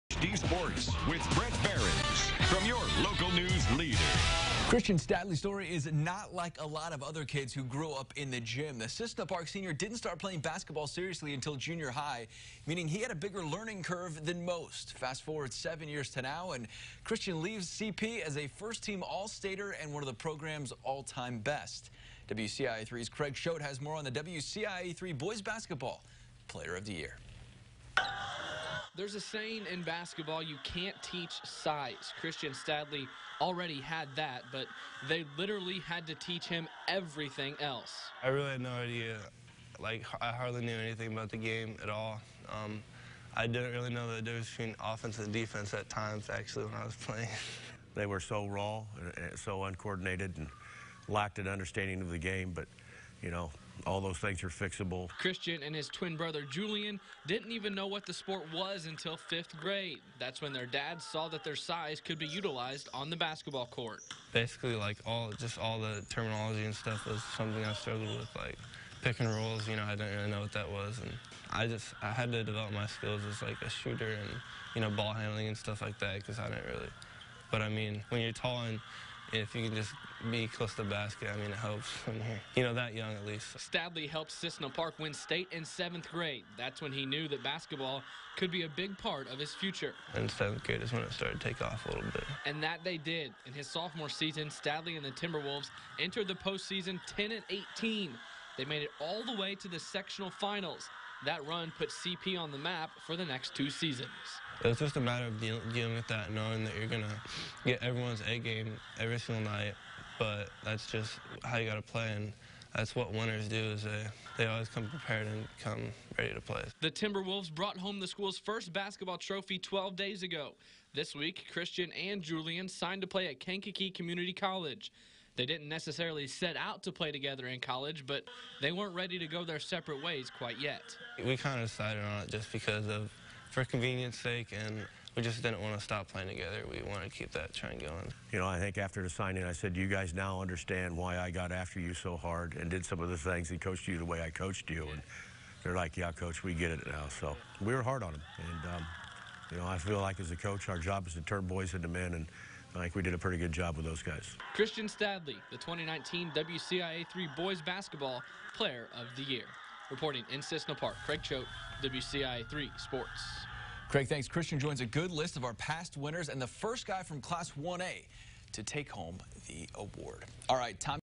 2019 WCIA 3 Boys' Basketball Player of the Year: Christian Stadeli
CISSNA PARK (WCIA) -- There's a saying in basketball... 'You can't teach size.' Christian Stadeli already had that, but they literally had to teach him everything else. WCIA 3's Craig Choate reports on the 2019 Boys' Basketball Player of the Year and his rise to one of the state's best players.